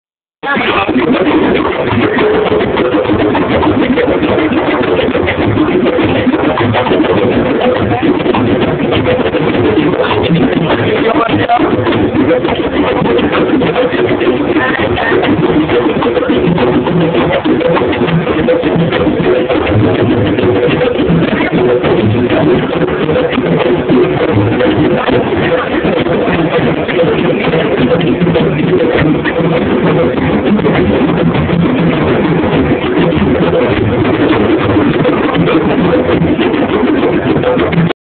Chicago fire jam
Look at that guy shake it! Oh and chopstick guy!